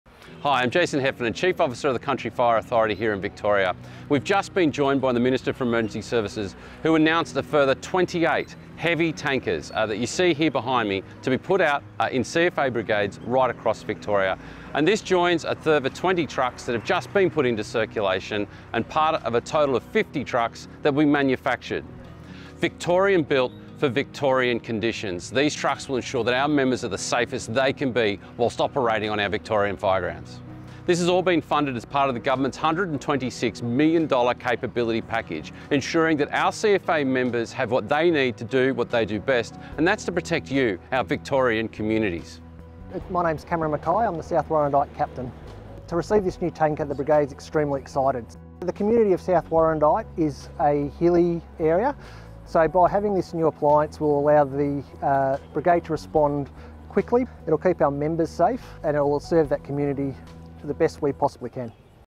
Heavy Tanker Launch
Today we announced the final 28 CFA brigades across the state that will receive replacement heavy tankers, ensuring our firefighters are well-equipped to keep Victorian communities safe.

We spoke to CFA Chief Officer Jason Heffernan and South Warrandyte CFA Captain Cameron Mackay about what this announcement means for our volunteers.

📍 Wurundjeri Country